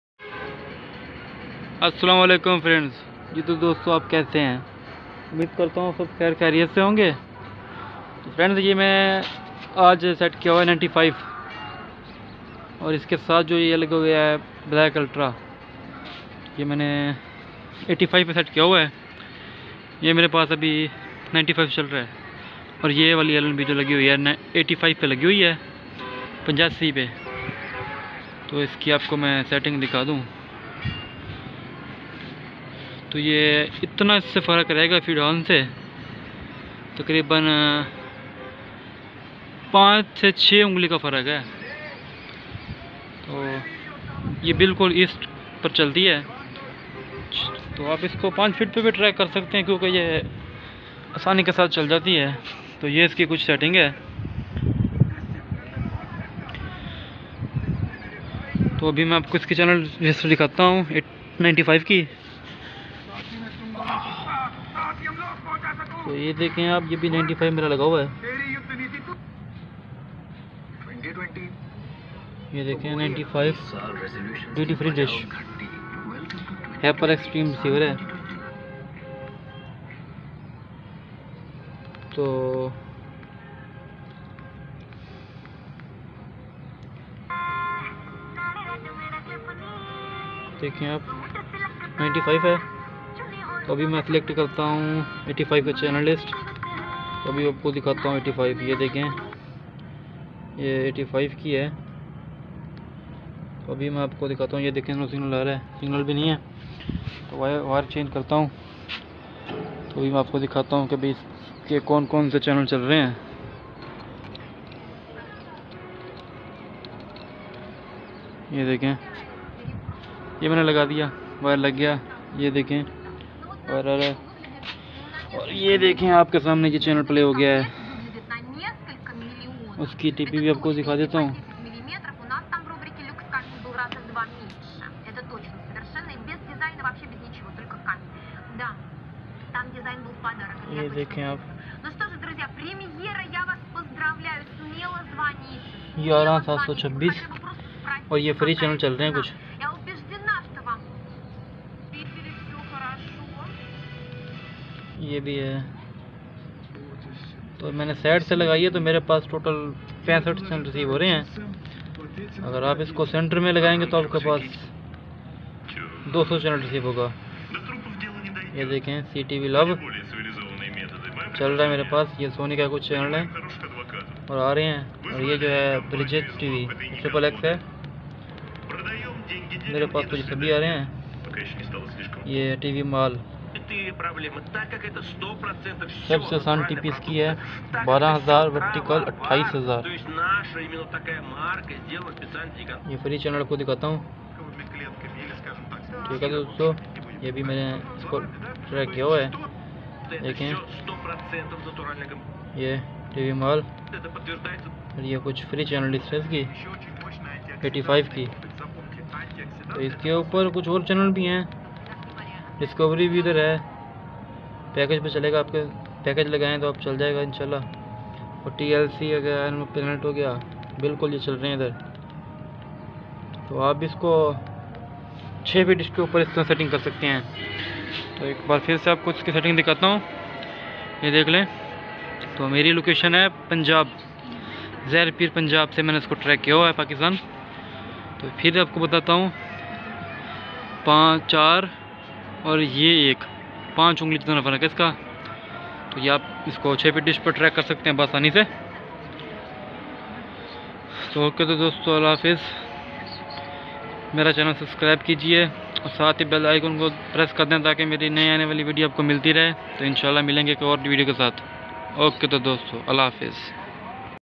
Today set NSS6 95 aur Horizon 2 85e on 6 feet dish Location zahir pirr Punjab
usama dish fitter
easy tp 95e 11170V27500
Horizon 2 easy tp 12000V28000